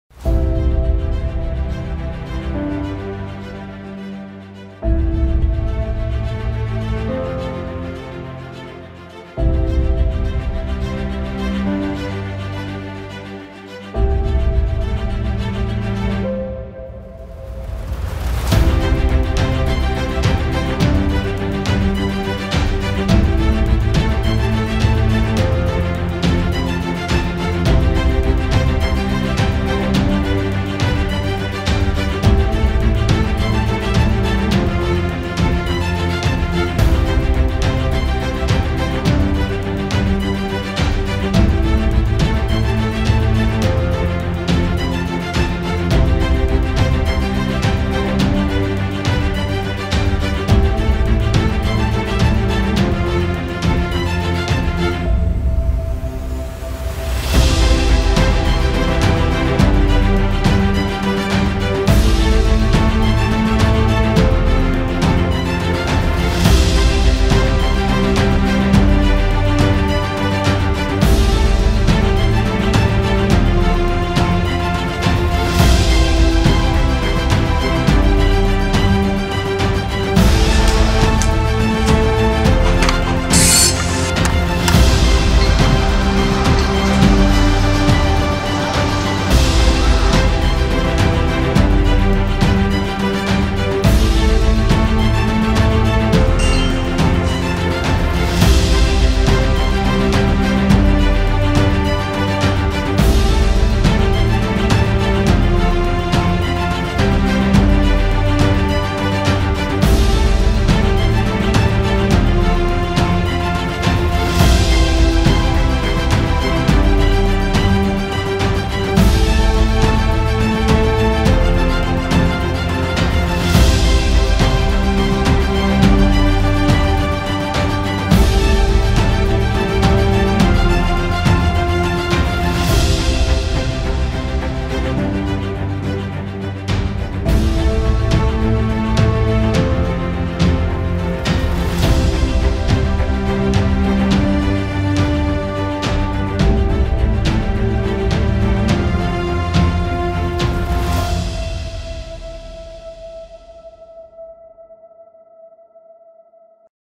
Vídeo Institucional MML Desenvolvimento Imobiliário Inteligente criado pela Forza em 2017
Em 2:37 minutos, o vídeo apresenta todo o posicionamento de marca e empresarial da construtora MML, de Passo Fundo. Com cenas captadas na construtora, nas obras e nas ruas e parques da cidade, aliadas a uma trilha desafiadora, a audiência logo percebe que se trata de uma empresa inovadora, atenta às tendências mundiais de muita credibilidade.
Produção: CNG e Forza